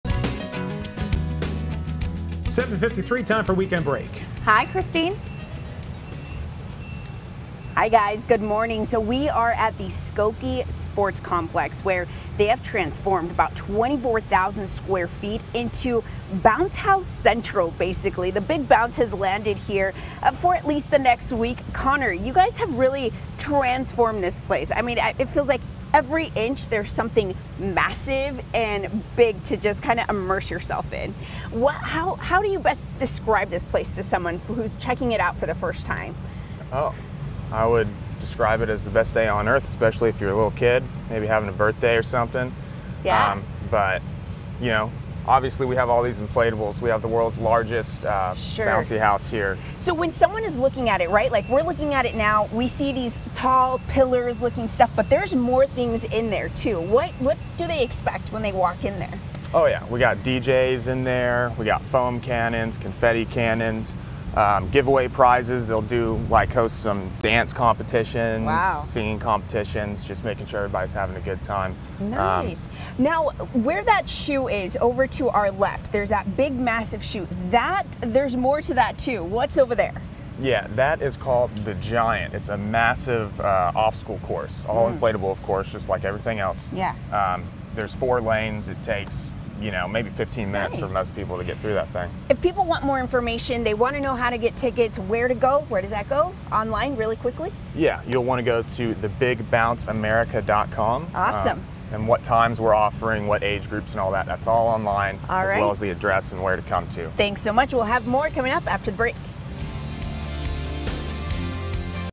Weekend Break: World's biggest bounce house in Skokie
WGN's Christine Flores takes you on a preview of the bounce house activities available at the Skokie Sports Park over Labor Day Weekend.